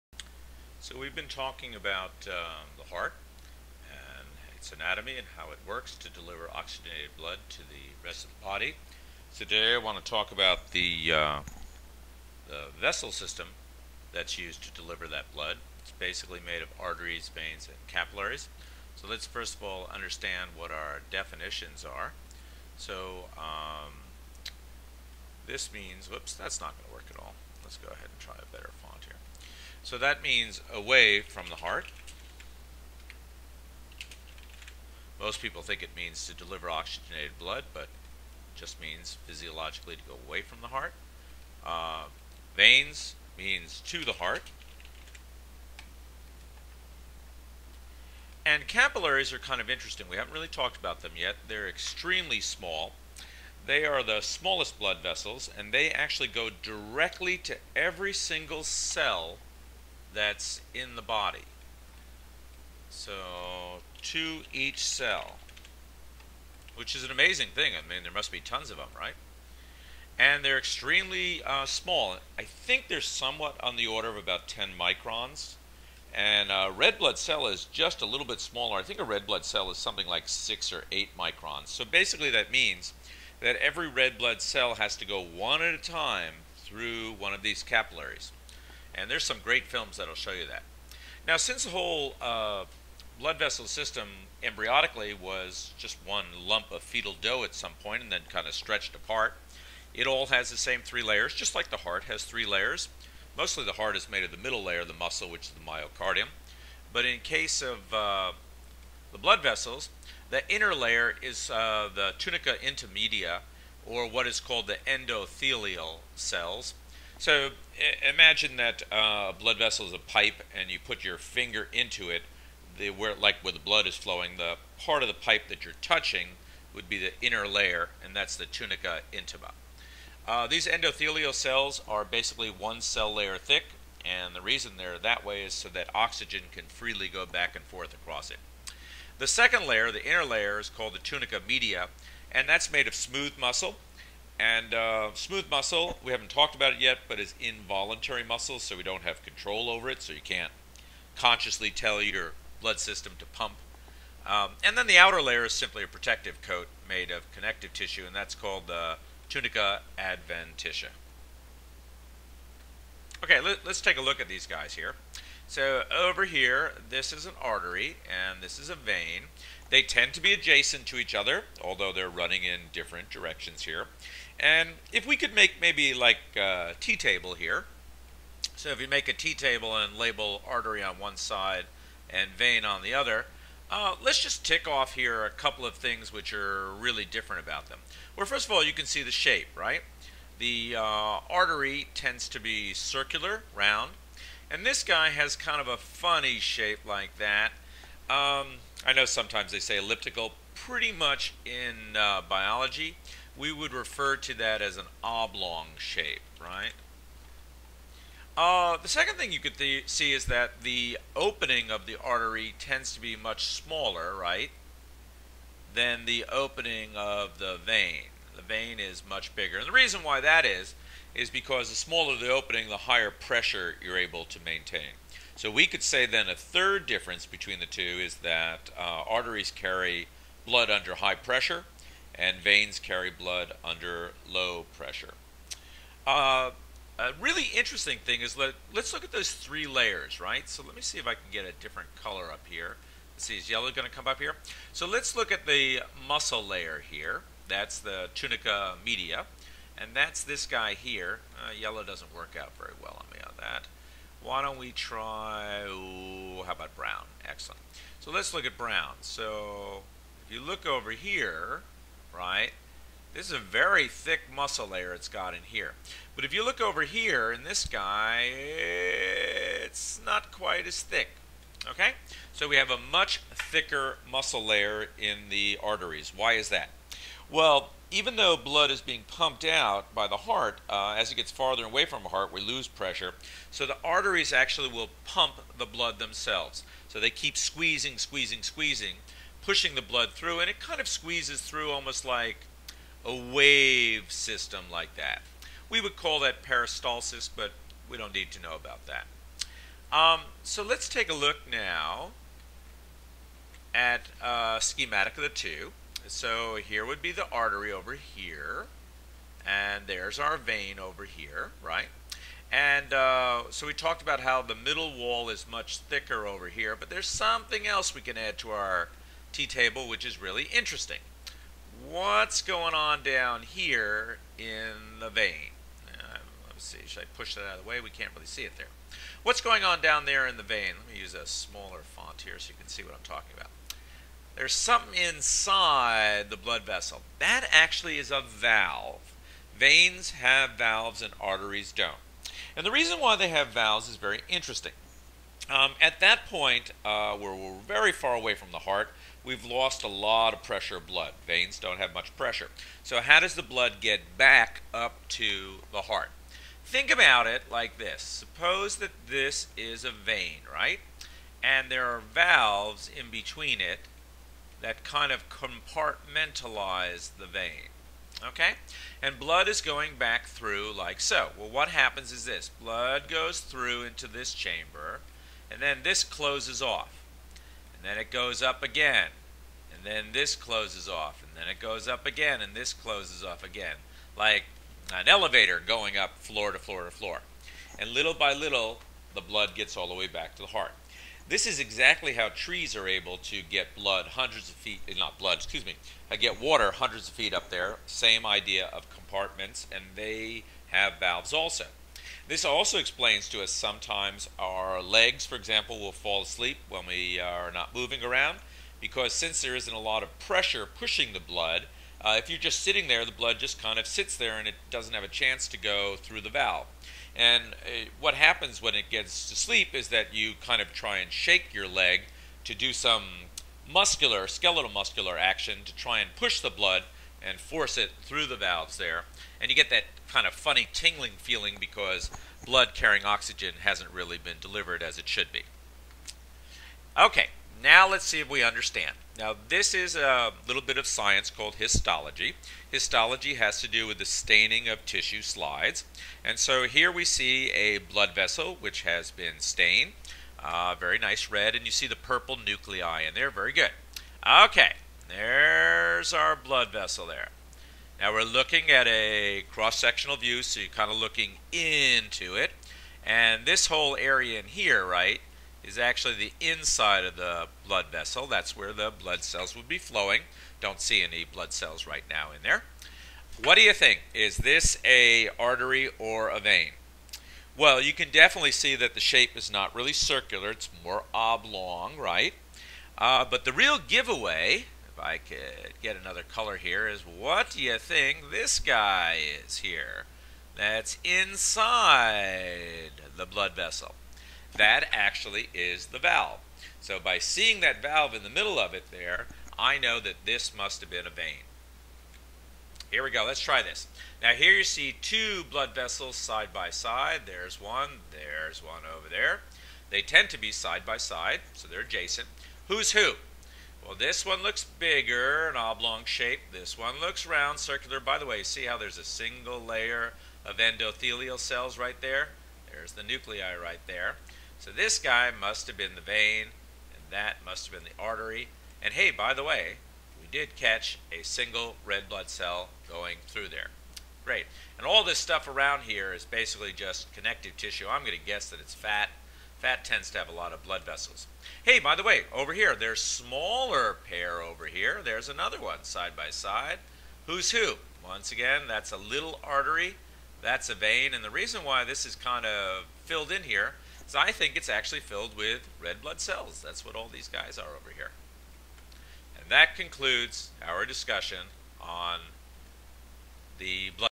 Blood Vessels
Winters explains the types of blood vessels to his IB Biology class